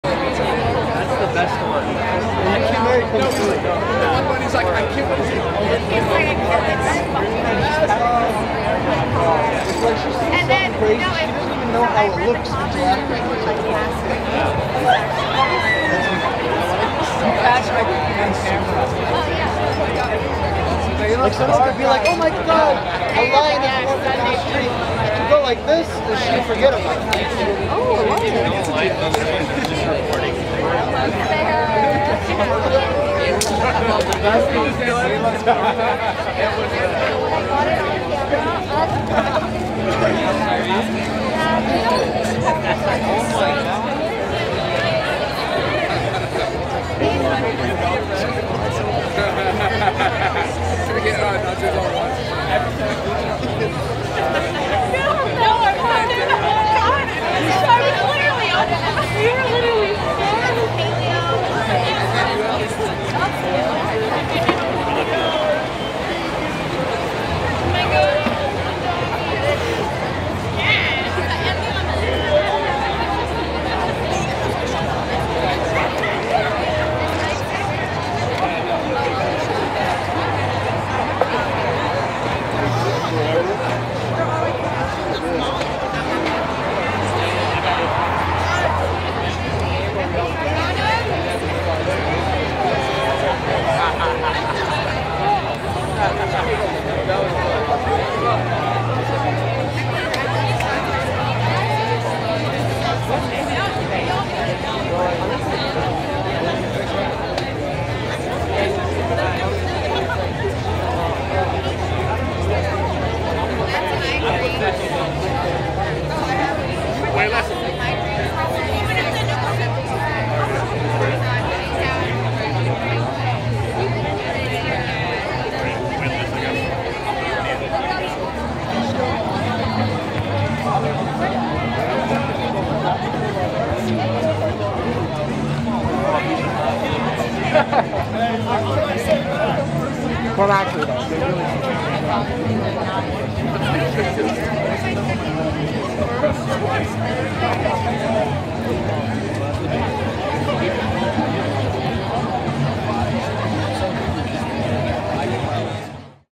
Blake Gray & Amelie Zilber Win The Cutest Couple Award While On A Dinner Date At Saddle Ranch
BlakeGray & AmelieZilber Win The Cutest Couple Award While On A Dinner Date At Saddle Ranch 9.1.20 - TheHollywoodFix